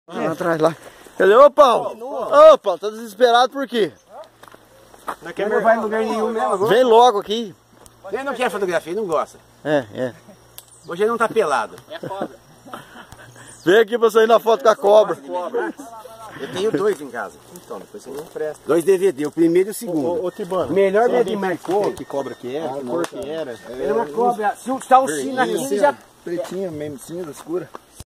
PedalAraraquara VD6 - SelfieRepresa
Passeio Incrível com a Galera do PedalAraraquara. Saímos a procura do Canão e encontramos represa rompida, rio todo devastado pelas aguas e muito sol no lombo. Um dos passeios mais incriveis que esse grupo já fez, com imagens inesquecíveis e uma galera muito unida do PedalAraraquara.